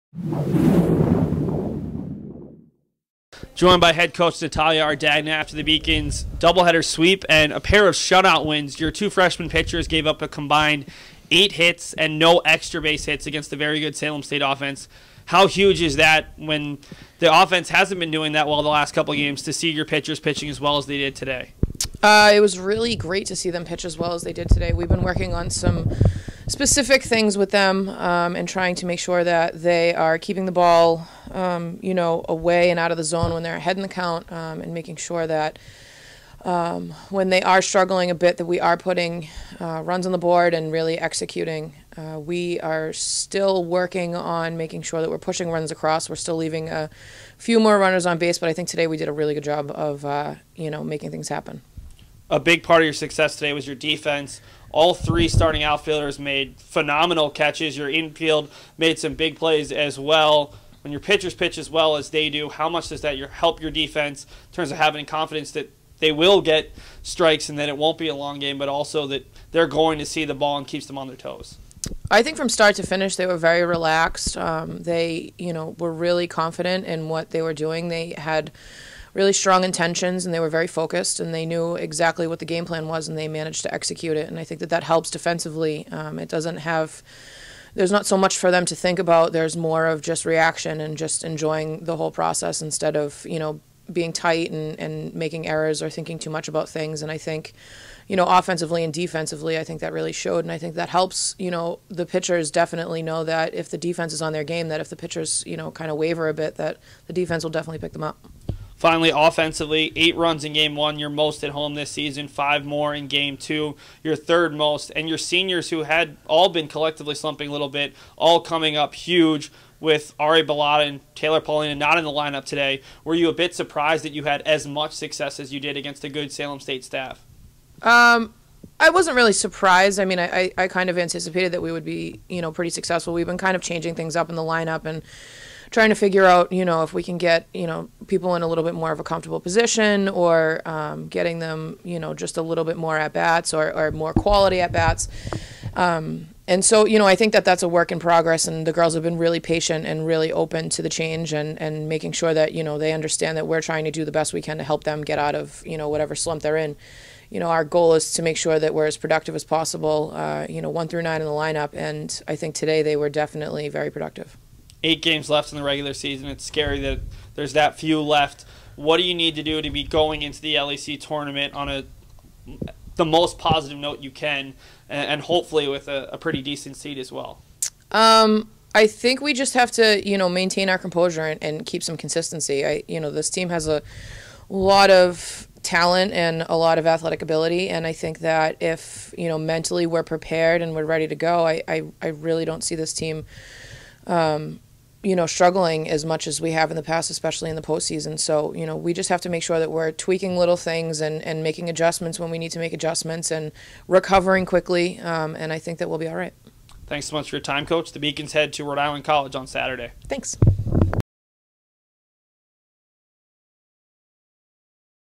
Softball Head Coach Natalia Ardagna Postgame Interview vs Salem State (4/20/17)
UMass Boston Softball head coach Natalia Ardagna talks about her team's first home doubleheader sweep of the season. The Beacons defeated Salem State University 8-0 (5 innings) and 5-0.